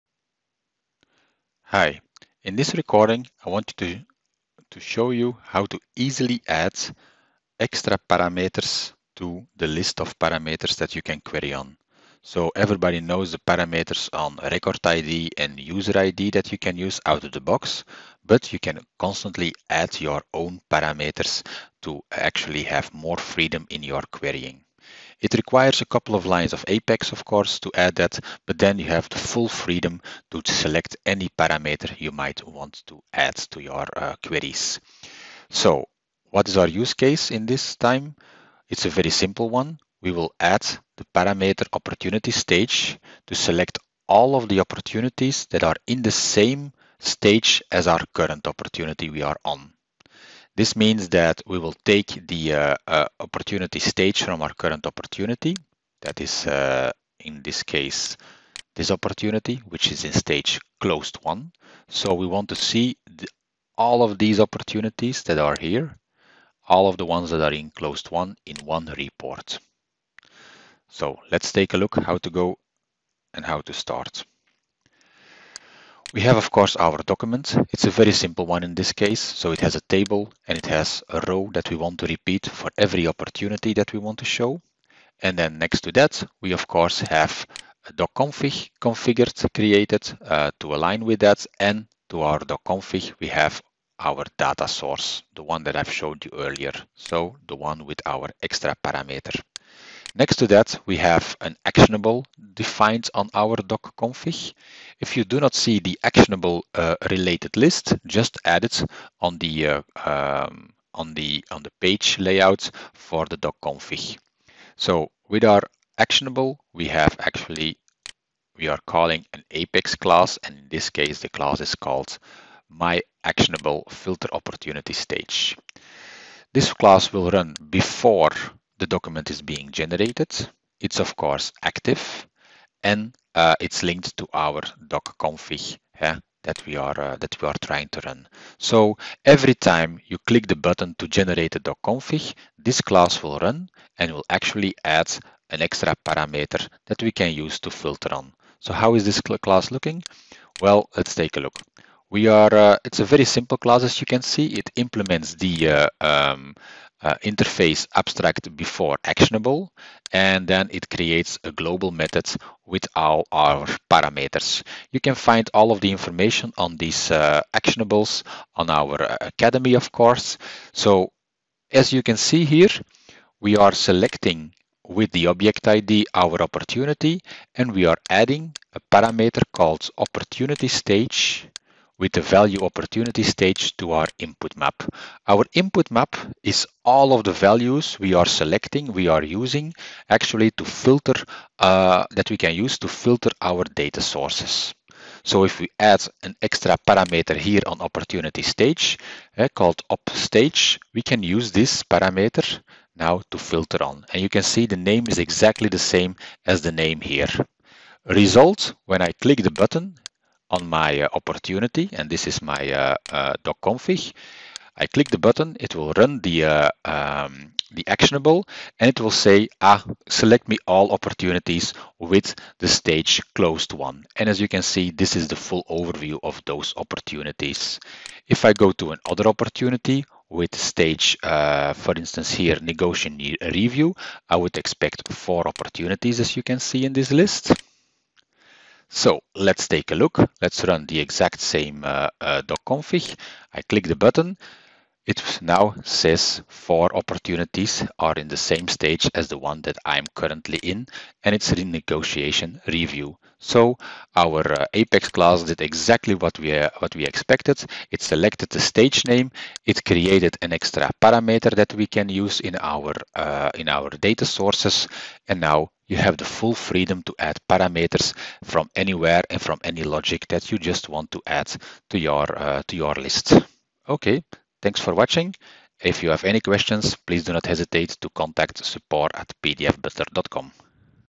Add SOQL Parameter By Actionable
Many cases, you want to use data from un-related sObjects or from screen input to filter your DataSources. PDF Butler allows to add parameters that can be used in your DataSources.